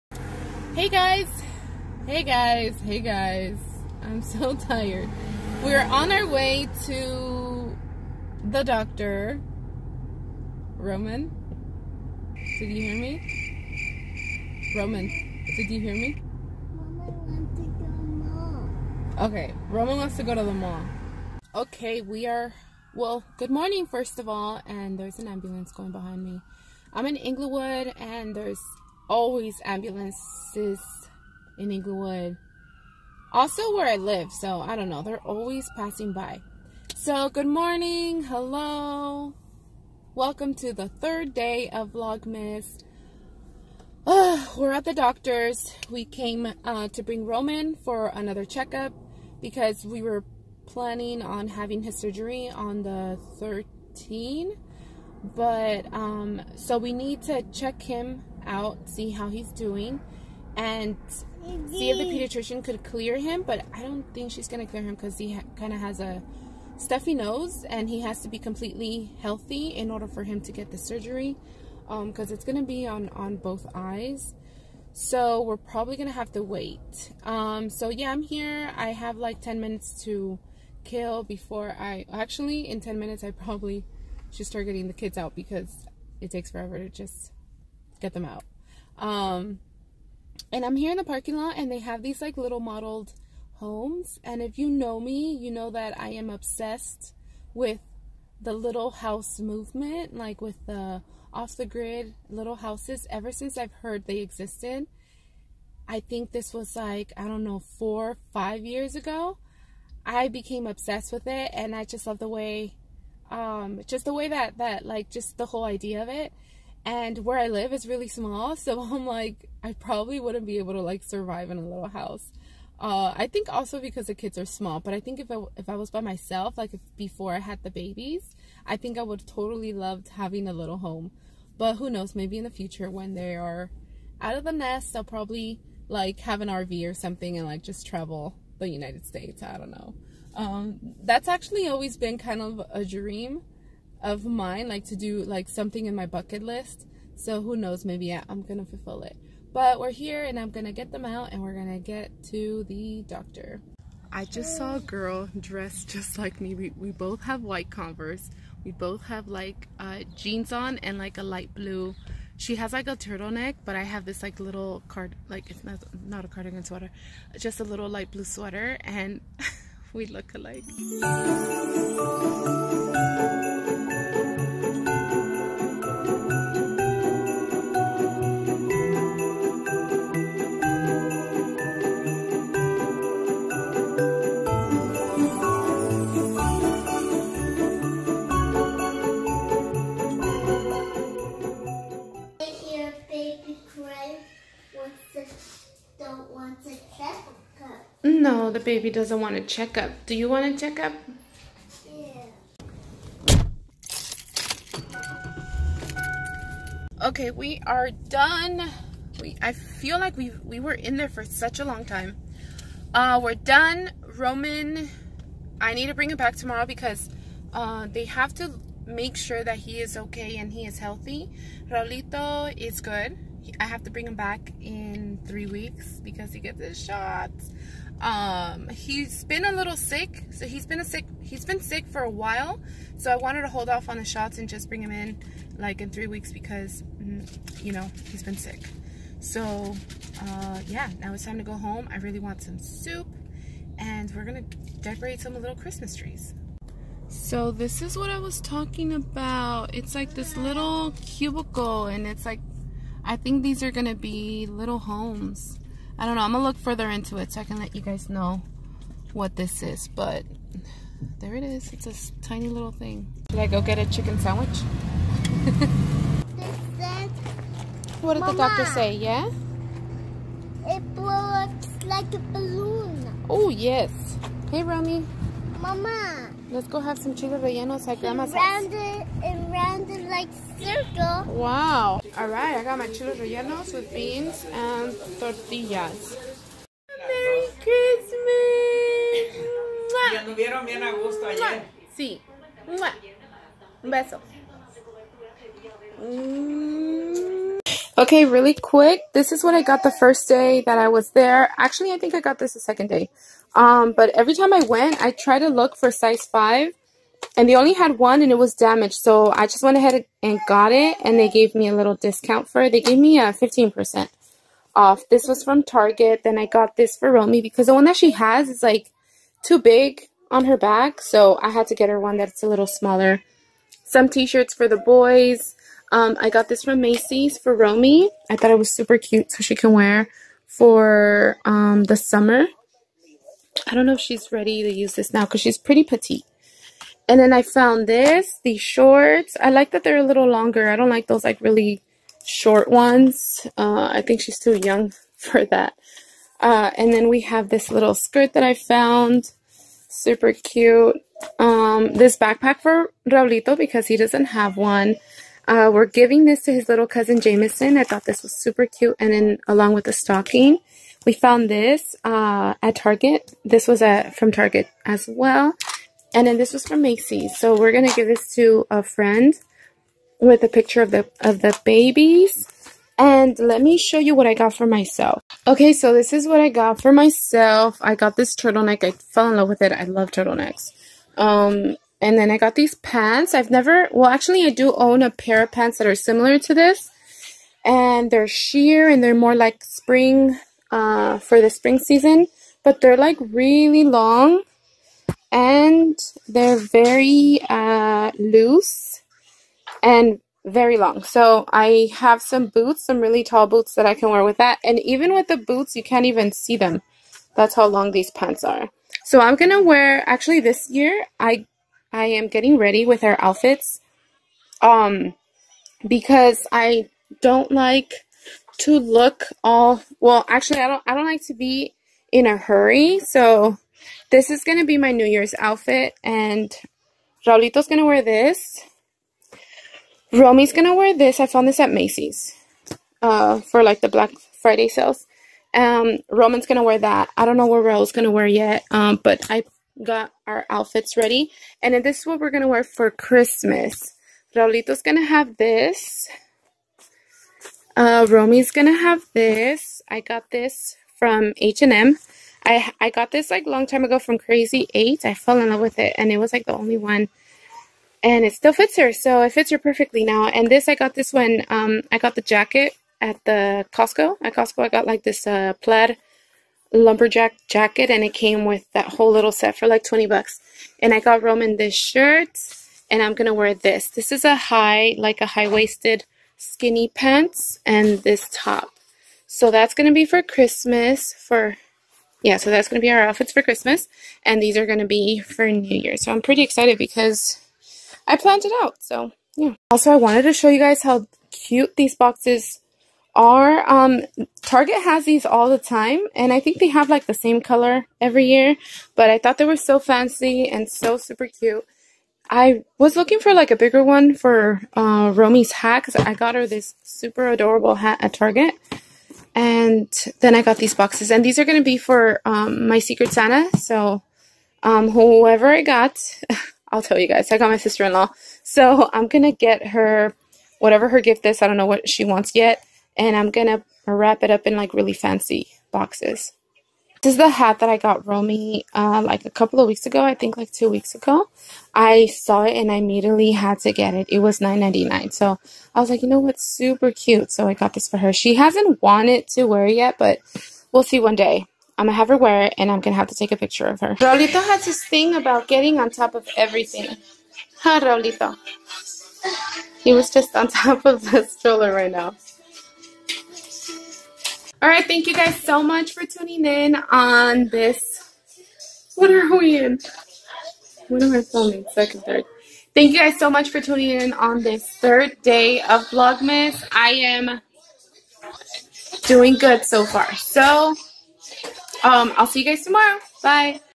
Collective Haul | Outfits for Christmas And New Years Ready!
hey guys!
here I am again with another vlog . took the kids to the doctor. Some chick was dressed just like me 😆 I show you guys the outfits I have ready for Christmas and New Years 🎉

snap | rrrosita